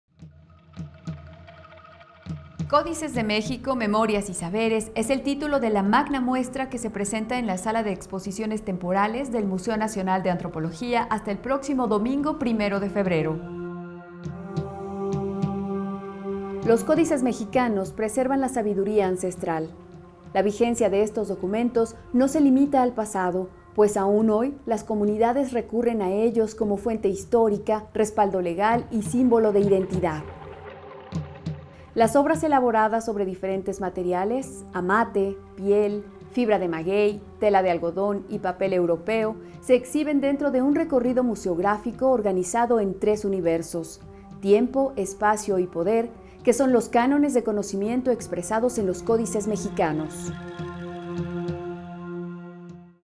Códices de México, memorias y saberes.
Códices de México, memorias y saberes es el título de la magna muestra que se presenta en la Sala de Exposiciones Temporales del Museo Nacional de Antropología, hasta el  próximo 11 de enero.

Los códices mexicanos contienen y preservan la sabiduría ancestral. La vigencia de estos documentos no se limita al pasado, pues aún hoy las comunidades recurren a ellos como fuente histórica, respaldo legal y símbolo de identidad.